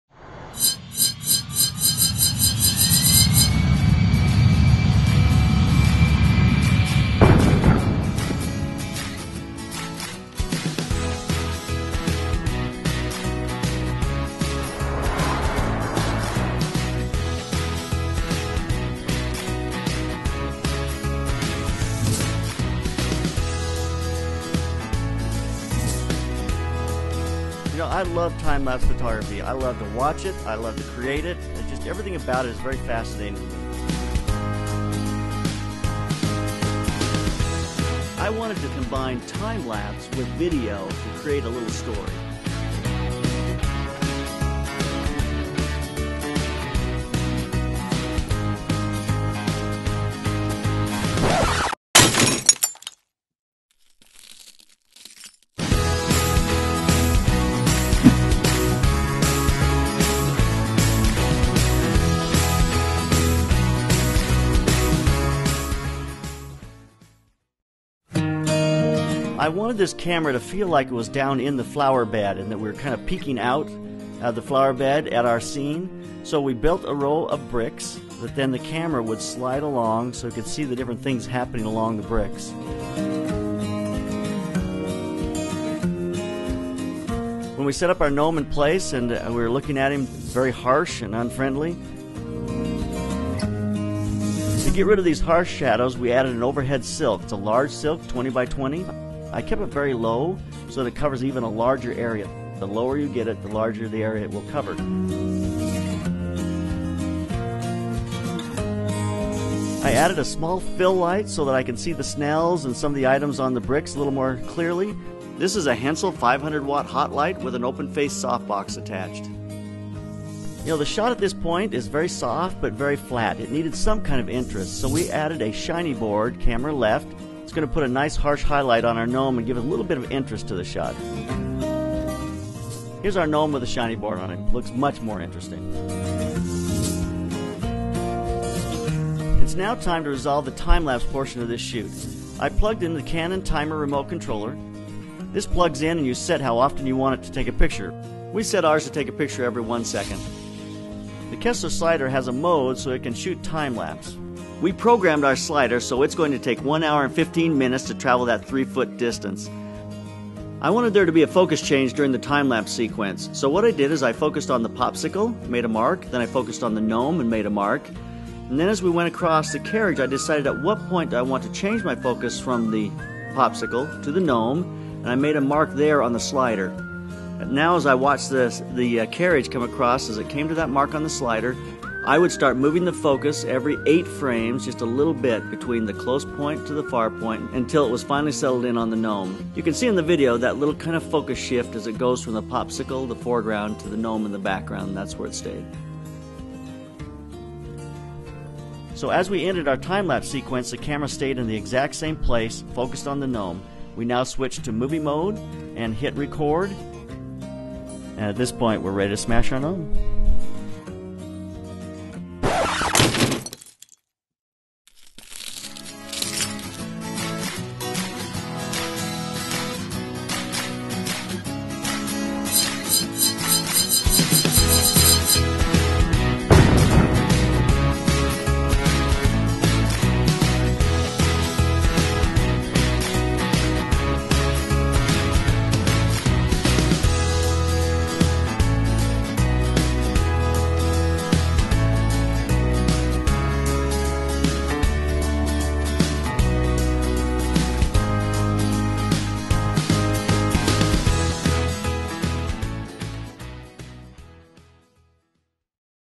Creating Time Lapse Photography: A Lighting Lesson
Shooting Time Lapse is one of the great adventures of still photography. In this lesson I will show how to shoot, and light a simple time lapse sequence. I wanted to shoot a time-Lapse piece in my style. I also combined the Time Lapse sequence with a live action piece as well. That made it even more fun. I used a Kessler Cinie slider that is programmable to make slow moves during time-laps shooting. The Gnome short was about 75 minutes to make the 3 foot move. This technology has come so far in the last 2 years. It was great fun. I hope you find it educational.